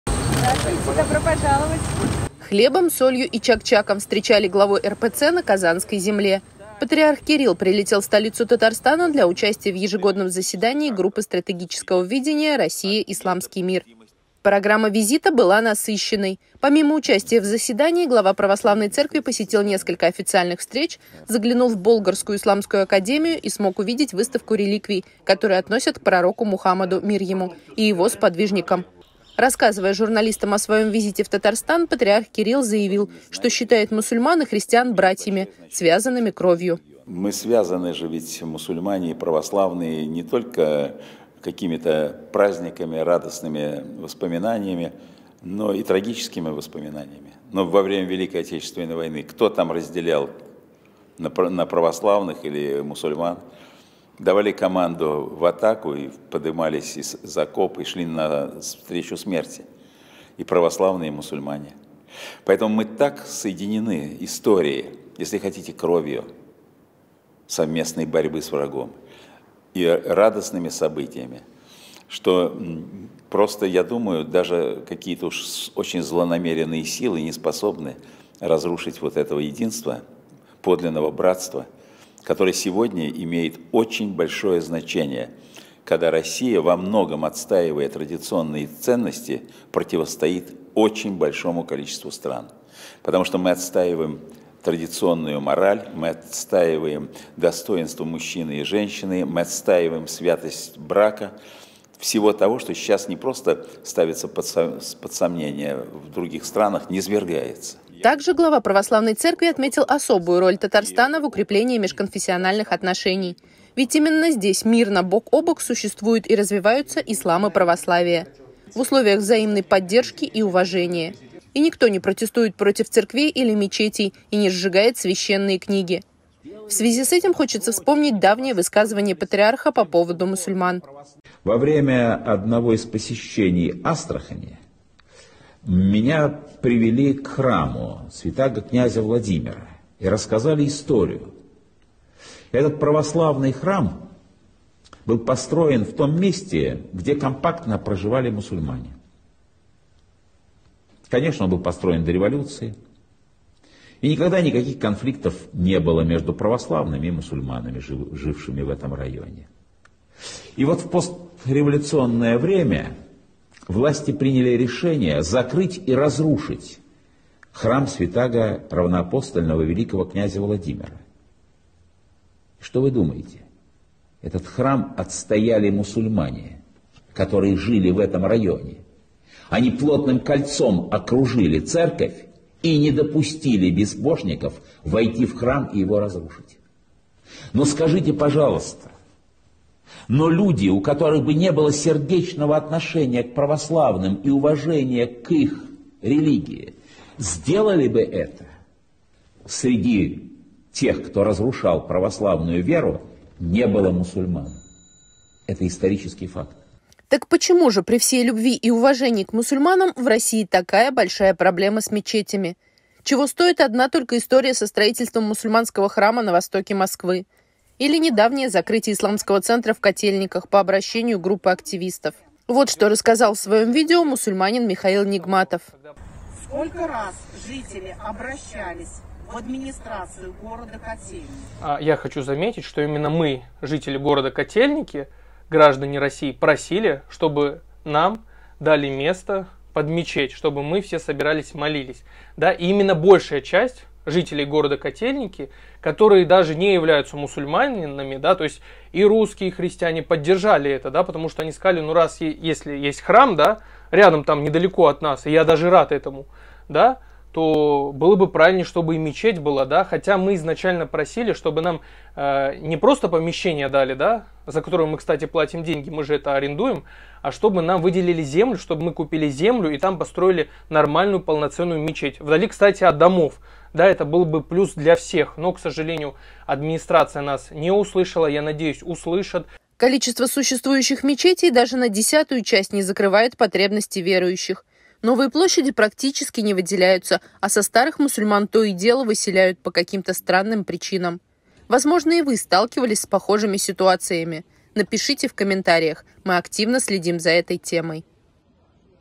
Неожиданно! Патриарх Кирилл высказался о мусульманах
Хлебом, солью и чак-чаком встречали главу РПЦ на казанской земле. Патриарх Кирилл прилетел в столицу Татарстана для участия в ежегодном заседании Группы стратегического видения «Россия – Исламский мир».
Встречаясь с журналистами высокопоставленный священнослужитель сделал интересное заявление.
Братья и сестры, поддержите исламский информационно-просветительский канал НУР ТВ!
Наши реквизиты: Карта Сбербанка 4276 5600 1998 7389 (MARLEN MAKHMETOV)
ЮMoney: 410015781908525